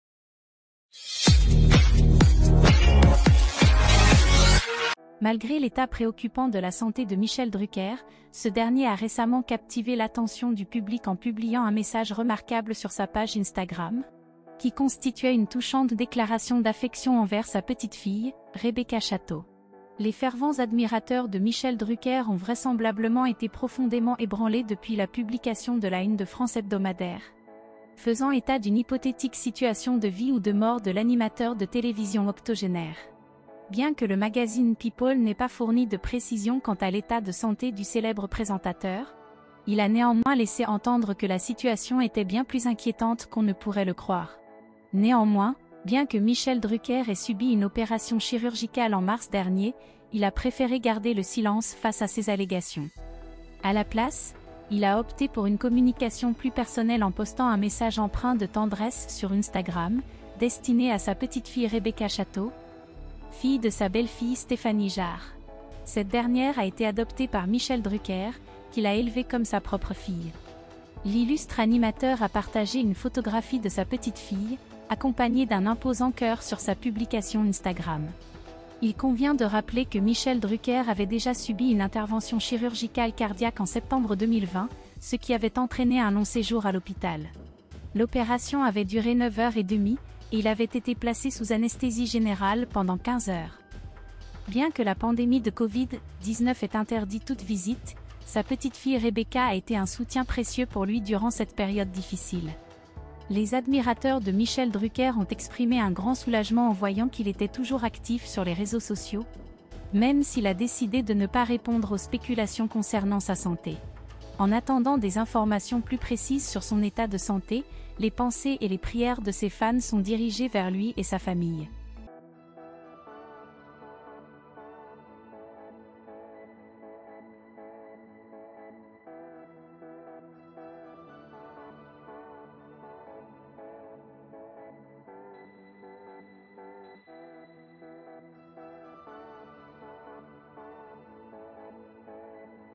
Michel Drucker : Malgré l'état préoccupant de l'animateur, une bouleversante déclaration
Malgré l'état préoccupant de la santé de Michel Drucker, ce dernier a récemment captivé l'attention du public en publiant un message remarquable sur sa page Instagram, qui constituait une touchante déclaration d'affection envers sa petite-fille, Rebecca Château.

Les fervents admirateurs de Michel Drucker ont vraisemblablement été profondément ébranlés depuis la publication de la Une de France Hebdomadaire, faisant état d'une hypothétique situation de vie ou de mort de l'animateur de télévision octogénaire. Bien que le magazine people n'ait pas fourni de précisions quant à l'état de santé du célèbre présentateur, il a néanmoins laissé entendre que la situation était bien plus inquiétante qu'on ne pourrait le croire.

Néanmoins, bien que Michel Drucker ait subi une opération chirurgicale en mars dernier, il a préféré garder le silence face à ces allégations. À la place, il a opté pour une communication plus personnelle en postant un message empreint de tendresse sur Instagram, destiné à sa petite-fille Rebecca Château, fille de sa belle-fille Stéphanie Jarre. Cette dernière a été adoptée par Michel Drucker, qui l'a élevée comme sa propre fille.

L'illustre animateur a partagé une photographie de sa petite-fille, accompagnée d'un imposant cœur sur sa publication Instagram. Il convient de rappeler que Michel Drucker avait déjà subi une intervention chirurgicale cardiaque en septembre 2020, ce qui avait entraîné un long séjour à l'hôpital. L'opération avait duré neuf heures et demie, et il avait été placé sous anesthésie générale pendant 15 heures. Bien que la pandémie de Covid-19 ait interdit toute visite, sa petite-fille Rebecca a été un soutien précieux pour lui durant cette période difficile.

Les admirateurs de Michel Drucker ont exprimé un grand soulagement en voyant qu'il était toujours actif sur les réseaux sociaux, même s'il a décidé de ne pas répondre aux spéculations concernant sa santé. En attendant des informations plus précises sur son état de santé, les pensées et les prières de ses fans sont dirigées vers lui et sa famille.